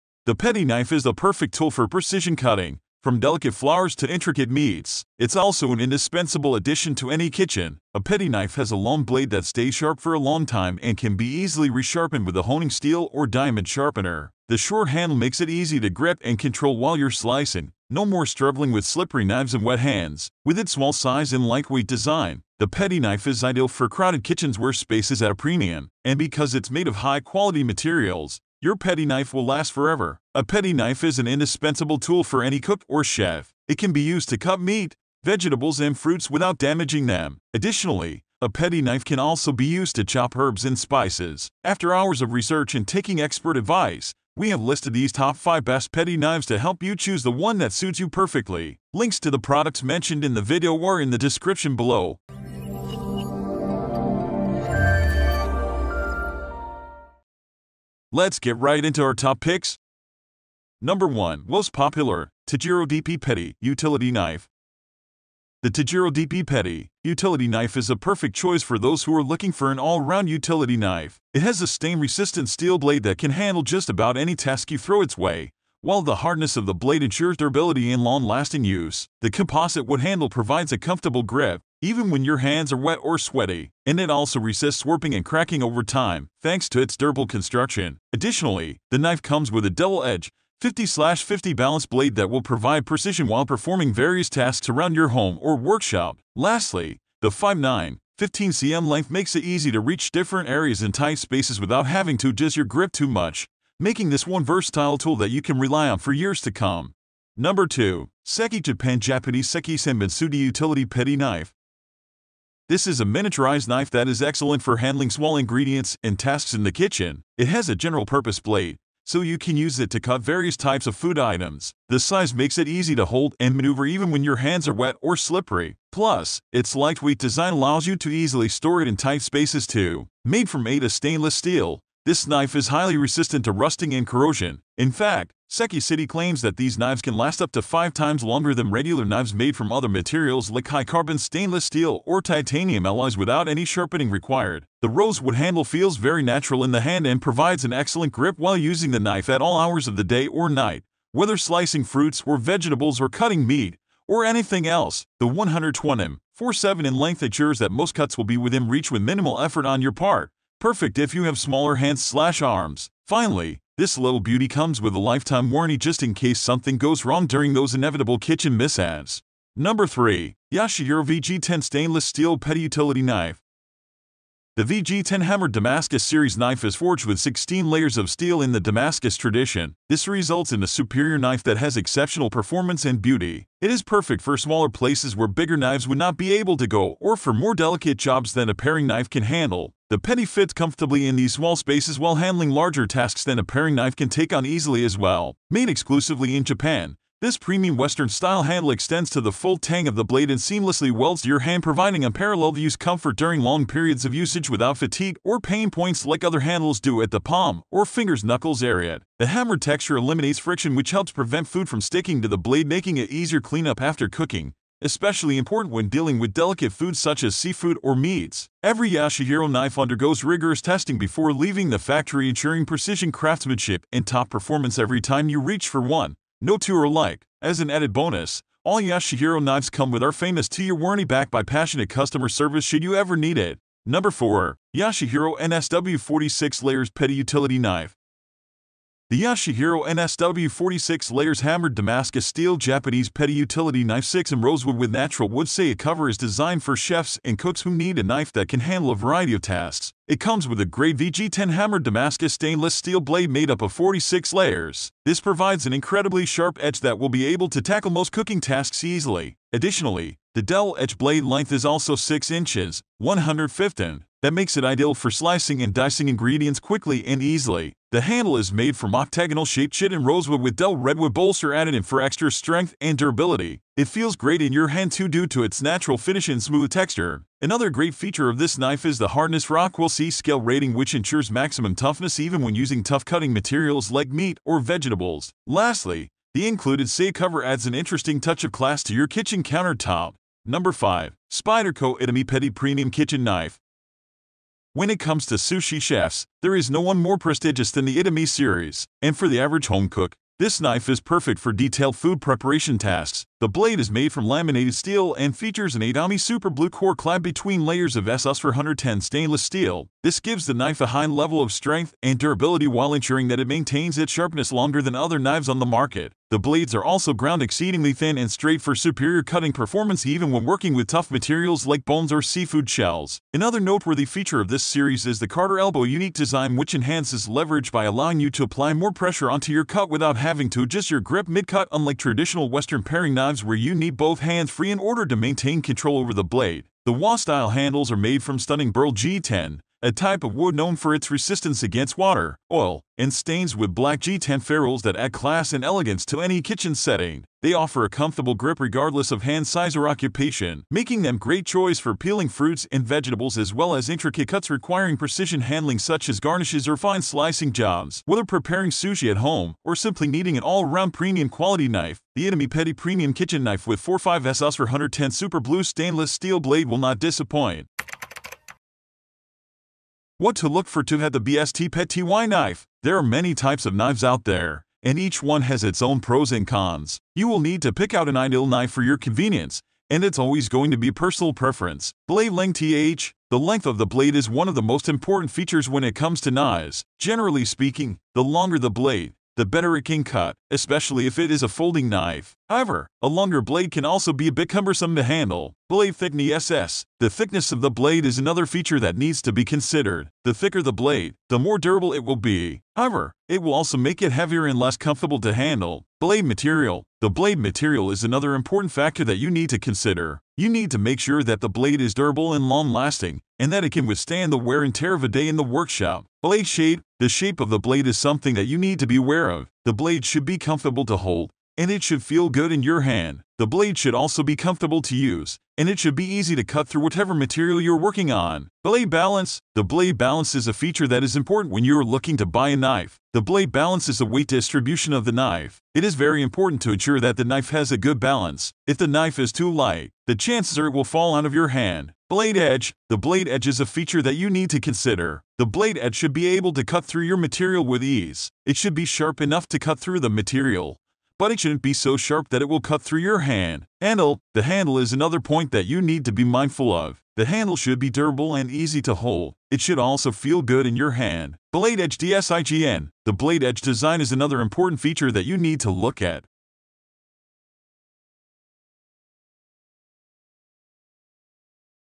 Top 5 Best Petty Knife Review in 2023 | Buying Guide
Best petty knife featured in this Video:
00:00 Introduction
01:01 NO. 1. Tojiro DP Petty/Utility Knife
01:47 NO. 2. Seki Japan Japanese Seki SANBONSUGI Utility Petty Knife
02:50 NO. 3. Yoshihiro VG-10 Stainless Steel Petty Utility Knife
03:54 NO. 4. Yoshihiro NSW 46 Layers Petty Utility Knife
04:51 NO. 5. Spyderco Itamae Petty Premium Kitchen Knife
06:08 Buying Guide

What is a petty knife?
A petty knife is a knife that is not as large as a standard knife, but is still large enough to be useful. They are typically used for smaller tasks, such as cutting vegetables or opening packages.

What is the best petty knife to buy?
If you are looking for the top petty knife, then you are in the right place.